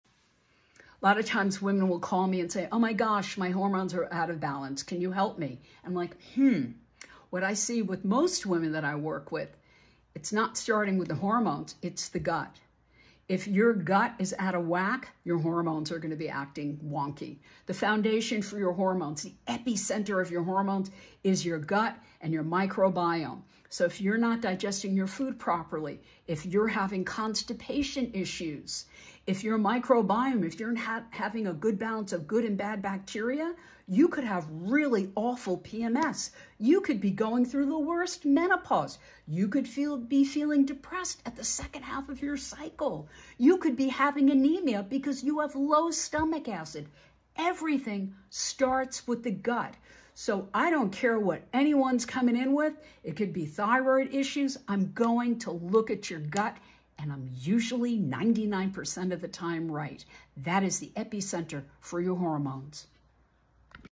Are your gut and hormones getting a divorce?
The gut is the epicenter for your hormones. If you're experiencing issues like pms, acne, thyroid problems, low energy, endometriosis, or one of many other mysterious ailments, you probably need to fix your gut.